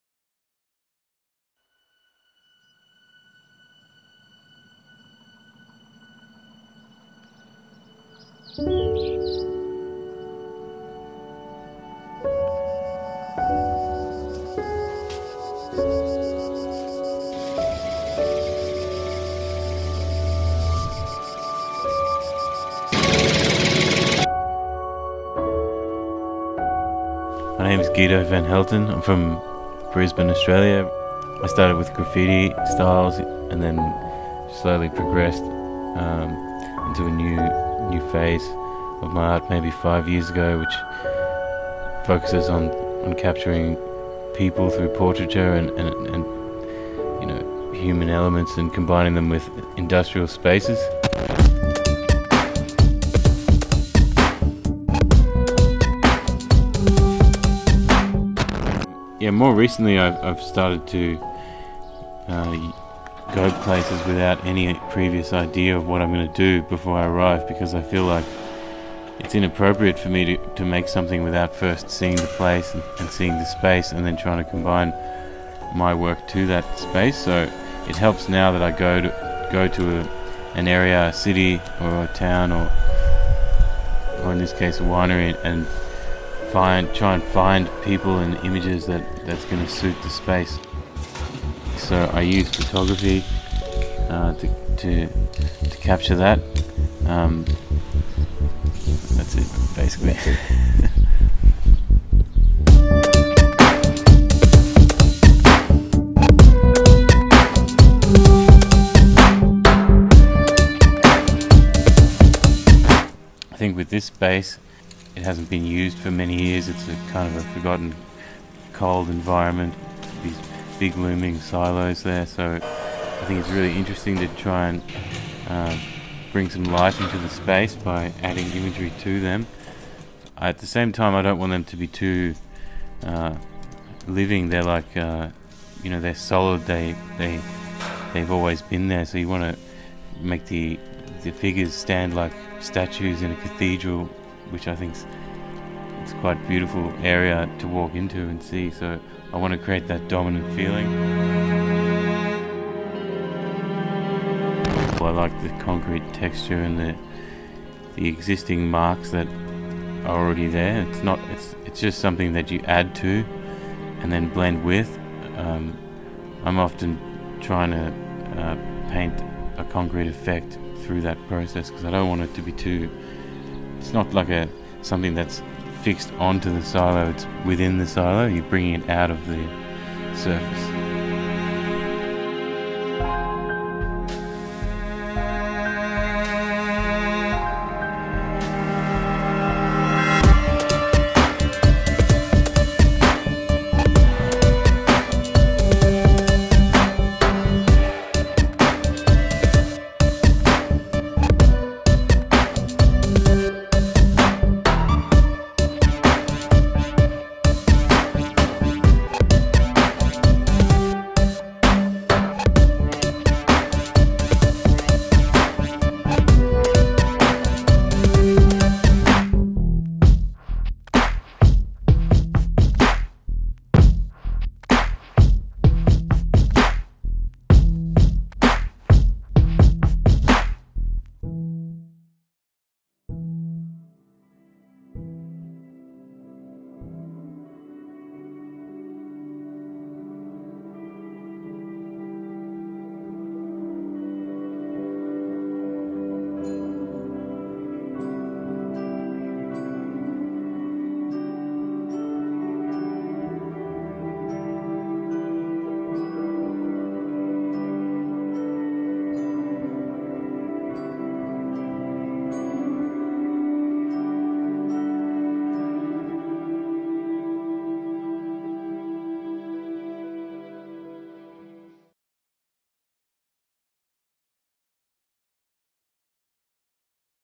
THE WINE CATHEDRAL by GUIDO VAN HELTEN
Guido van Helten travelled to Laguardia (Spain) and starts his beautiful project at Solar de Samaniego's winery.
Guido Van Helten’s artistic intervention in Laguardia is his first work in Spain.

It is the result of his interpretation of our project Beber Entre Líneas (Drink Between the Lines), the fusion between literature and the culture of wine.

The project is inspired by the vineyard La Escobosa, that back in the 18th century belonged to the renowned Spanish writer Félix María de Samaniego.

Directed, shot and edited by Edgar Lledó
Shot on Sony A7S